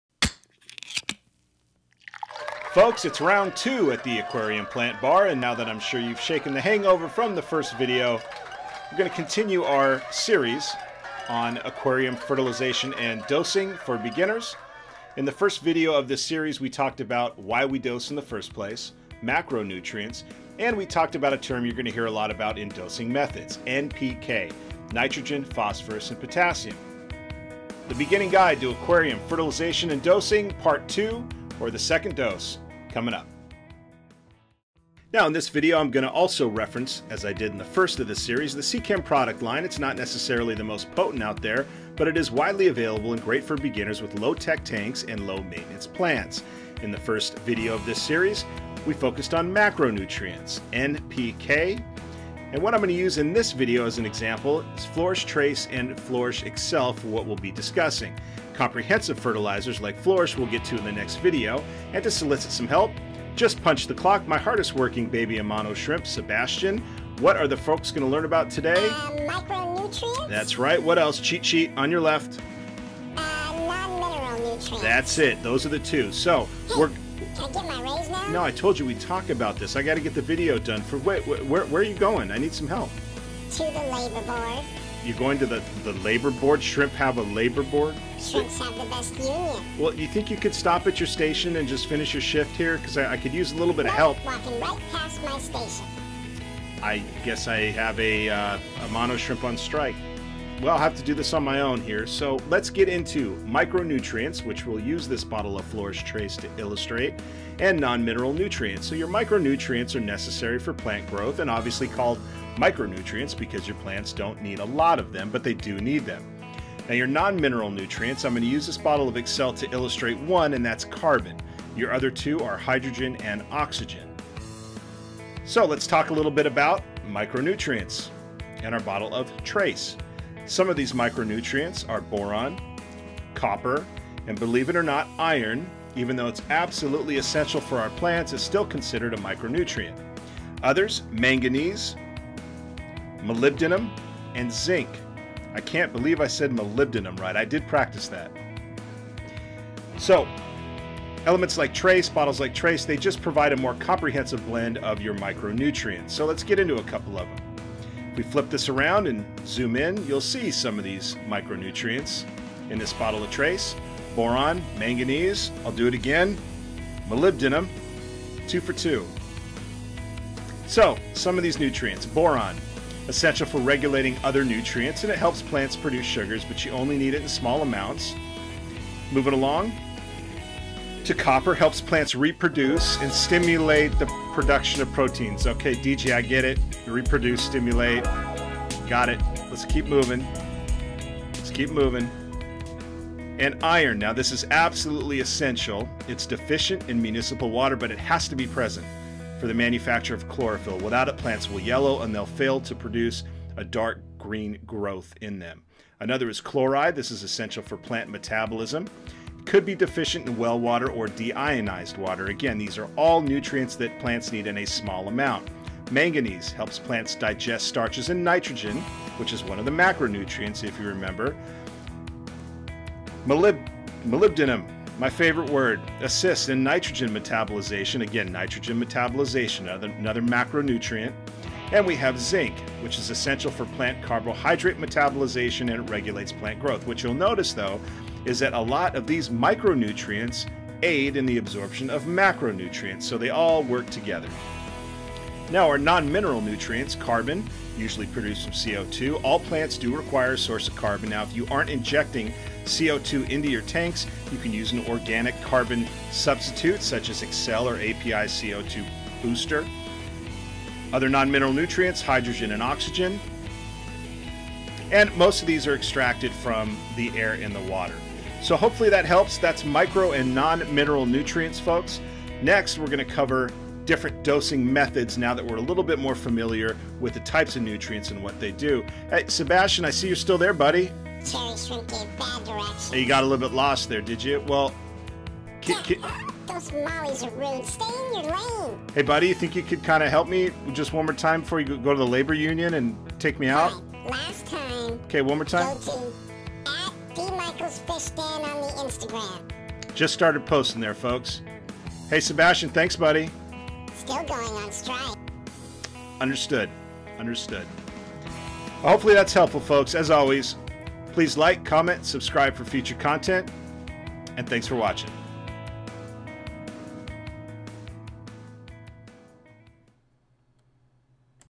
Beginning Guide To Aquarium Fertilization And Dosing....Part 2 - The Second "Dose."
Folks, I hope this video series is proving helpful.  In the first video of this series, Beginning Guide To Aquarium Fertilization And Dosing, we discussed why we dose, and we talked about some of the macronutrients, namely, nitrogen, potassium, and phosphorus.

In this second video of the series, we will discuss micronutrients and non-mineral nutrients. And I almost had help from my hardest working baby Amano shrimp, Sebastian, or at least I thought I did.

Keep tankin!

--d